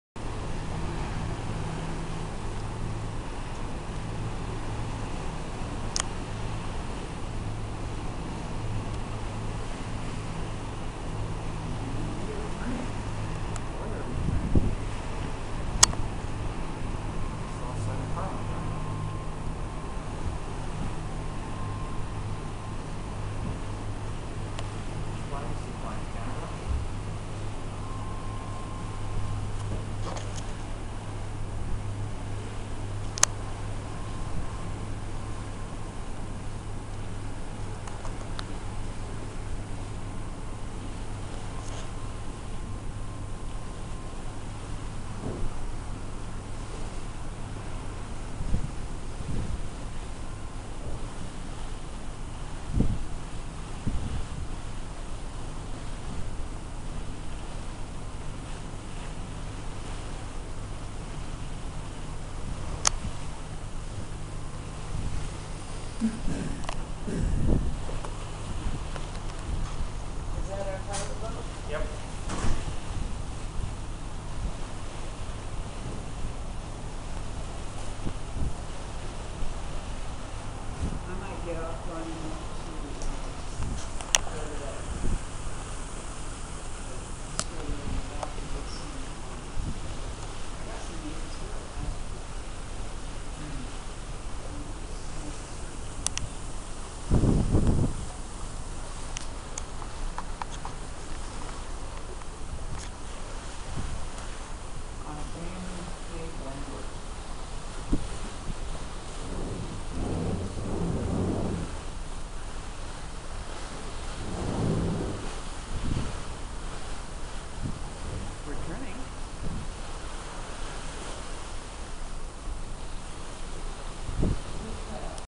Two boats meet near Victoria, BC
Seen on a cruise to Alaska.  Thursday, October 1, 2009